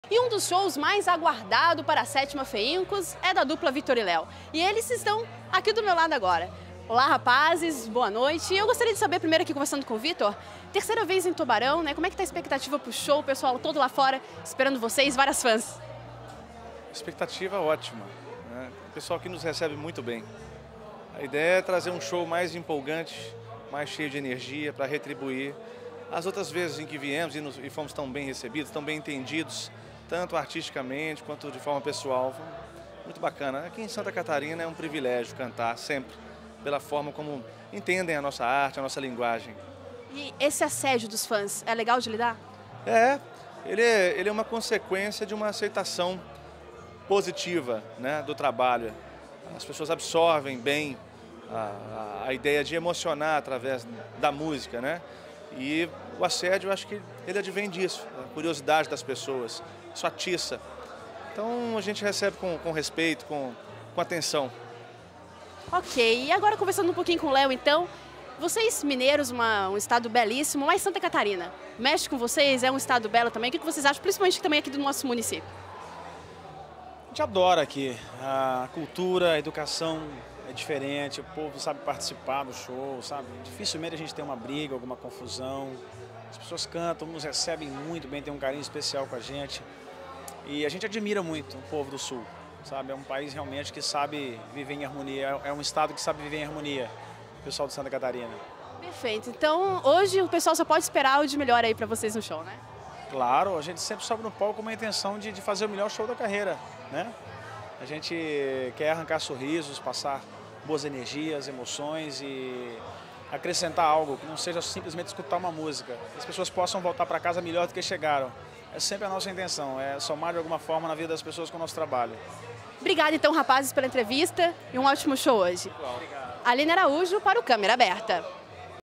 Victor e Léo falam da carreira e fãs
Um dos shows mais esperados do fim de semana aconteceu no sábado (22/5). Antes de subir no palco Victor e Léo conversaram com a repórter Aline Araújo